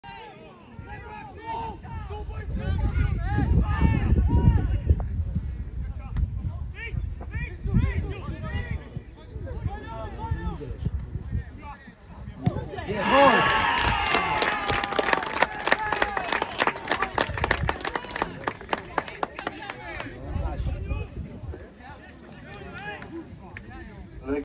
Legia II Warszawa 4-0 Broń Radom 16.03.2024 r. Julien Tadrowski gol na 1-0
Legia II Warszawa 4-0 Broń Radom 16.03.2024
21 kolejka III ligi 2023/2024, grupa I
Legia Training Center, Książenice
Julien Tadrowski 36’ gol na 1-0